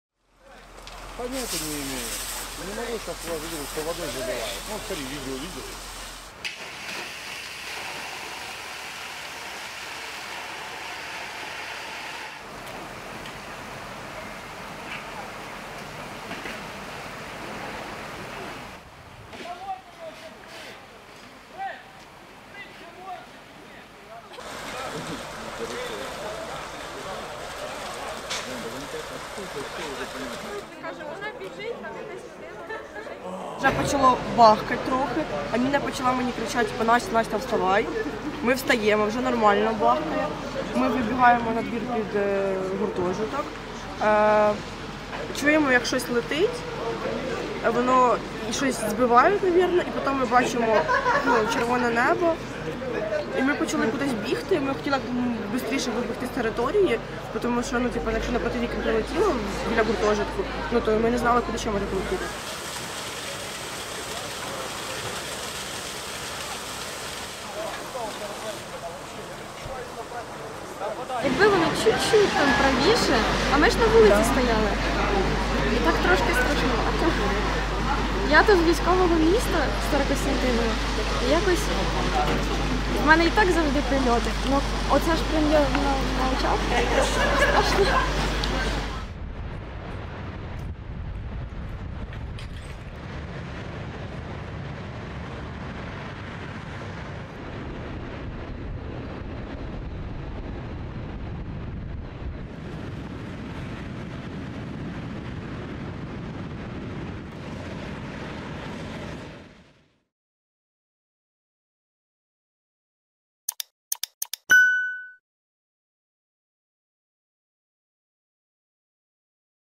Ukraine hit by Russian strike hours before children return to school
Firefighters work at a smoldering academic building in Kyiv after the latest barrage from Russia. Authorities say around 30 missiles were launched at the country, with 22 shot down by air defenses. The attack came just hours before Ukrainian children were due to start their third school year in the shadow of Russia's invasion.

AFP VIDEO